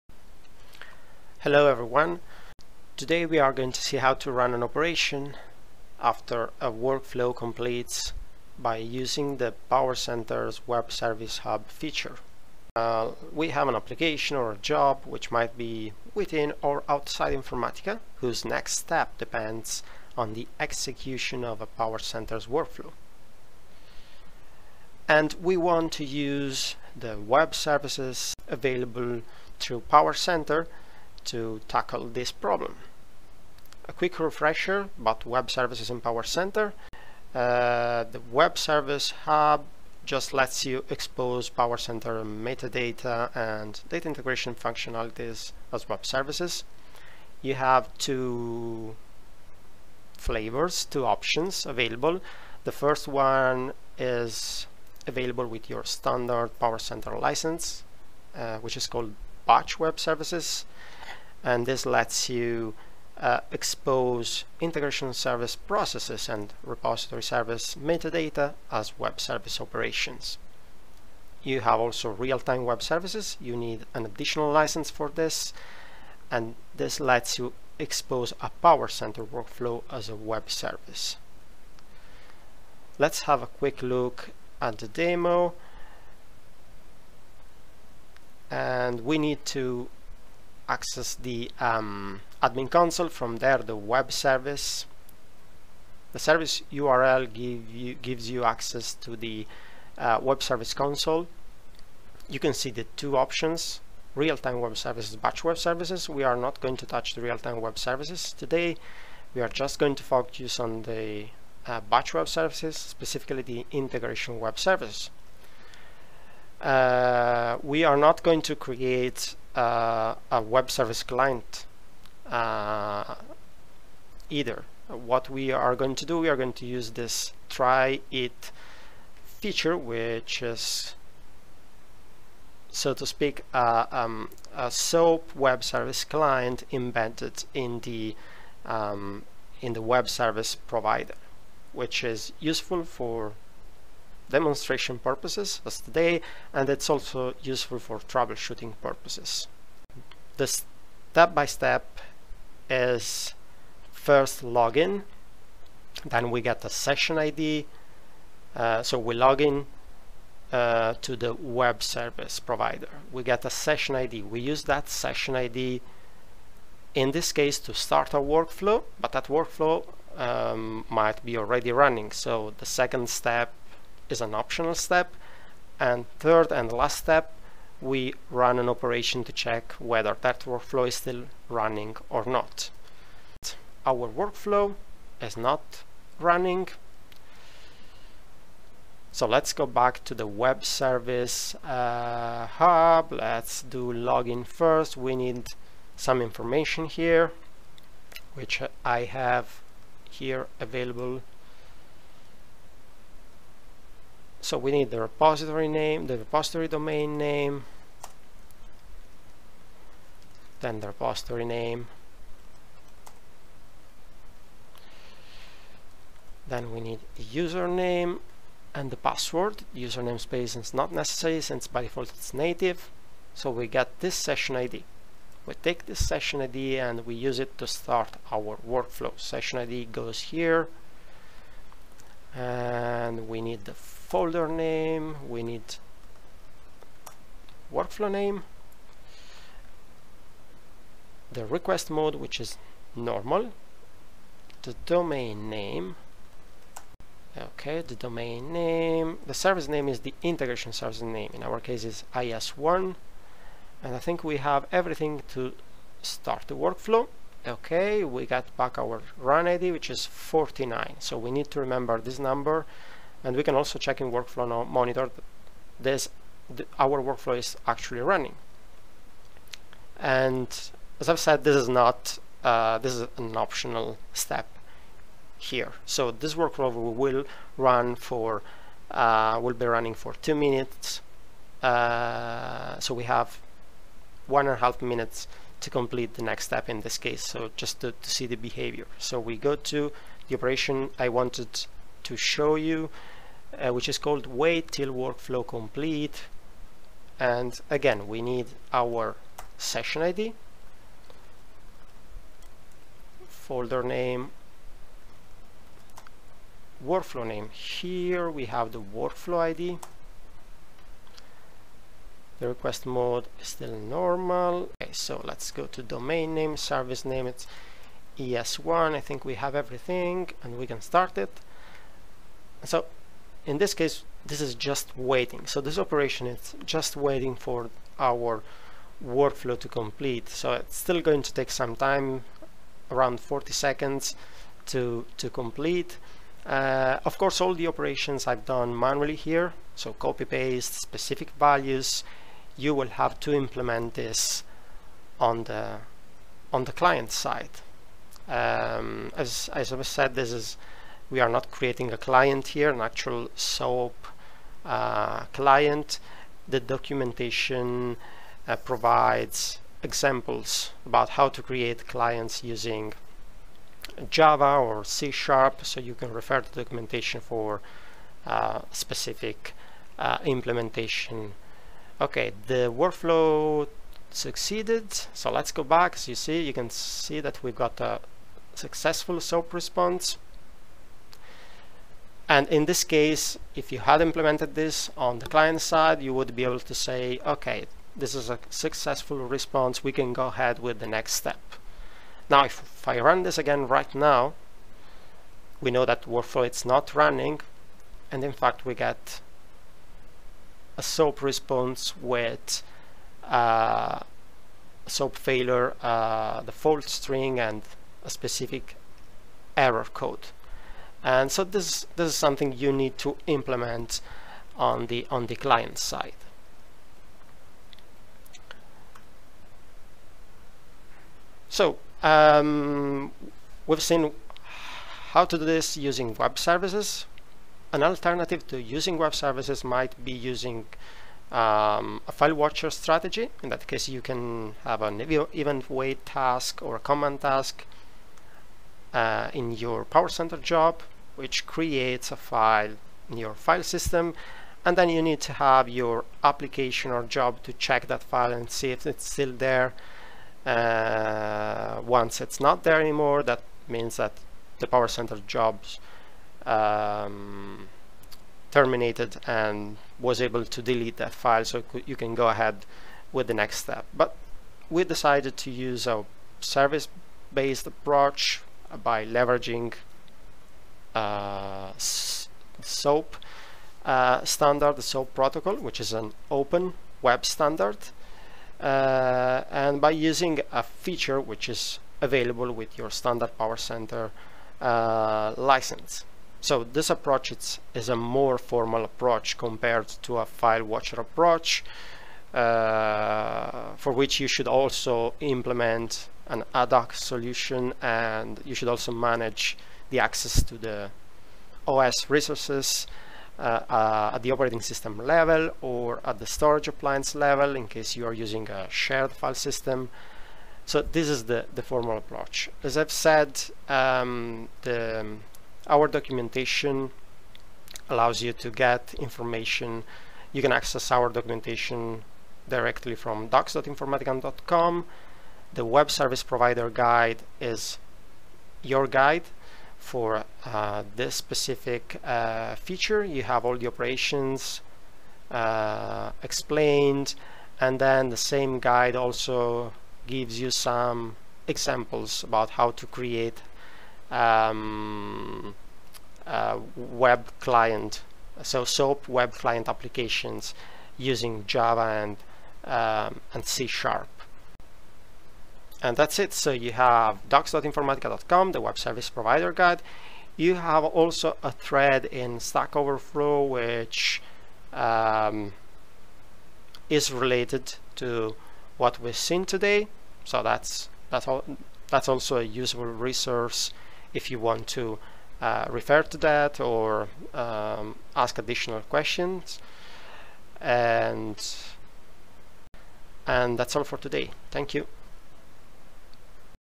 How to Run an Operation after a Workflow Completes in PowerCenter using Batch Web Services
This video explains how to run an operation after a workflow completes in PowerCenter using Batch Web Services.

StackOverflow - invoke Informatica PC wf from Java and read the response to resume java program

Expertise : Beginner
User Type : Administrator, Developer
Category : Product Feature
Project Phase : Implement
Problem Type : Product Feature